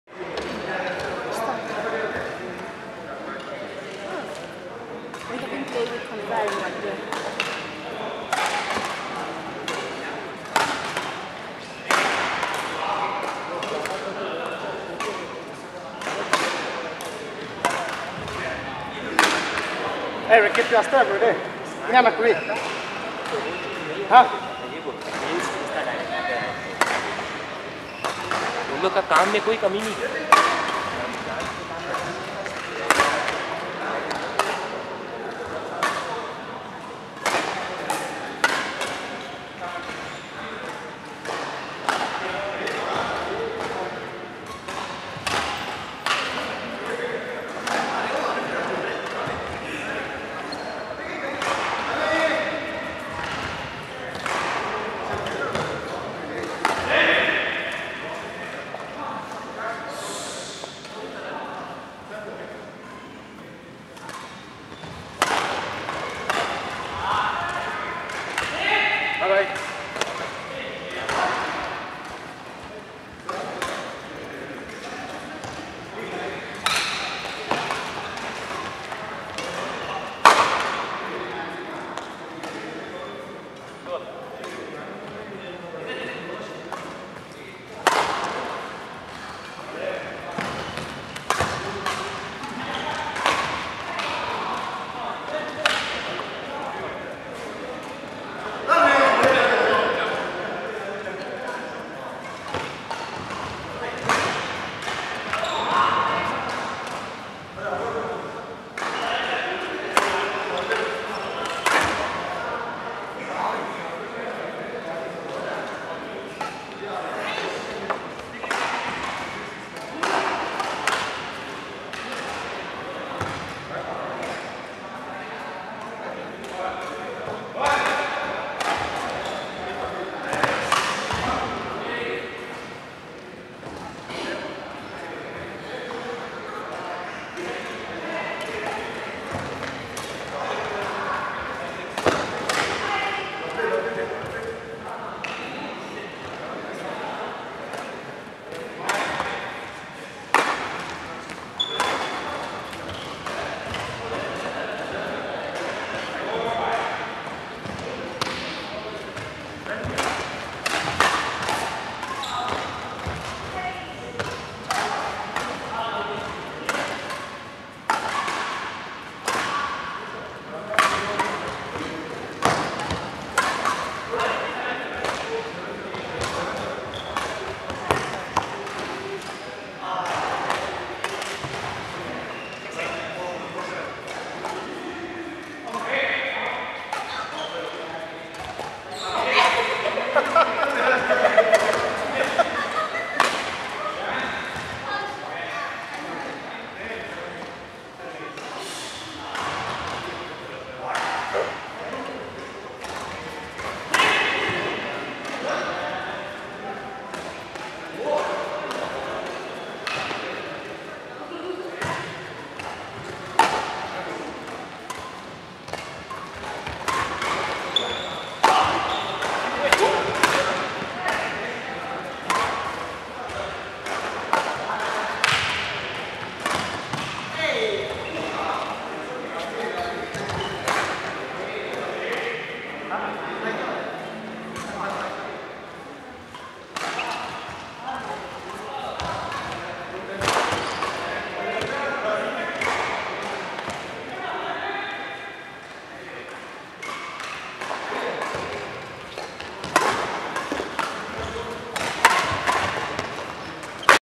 Pullela Gopichand Badminton Academy (clip 4)
25th March 2016. Having a fun time with friends in Gopichand Badminton Academy, Hyderabad